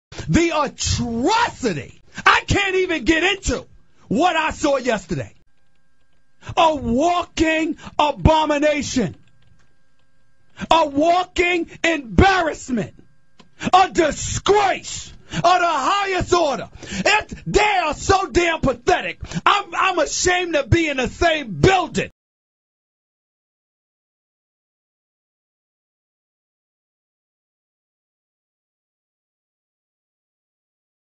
Michigan State 2012-13 basketball
MSU's Title dreams were shattered by Duke in the Sweet 16. **I OWN NO PHOTOS OR THE AUDIO** Enjoy!!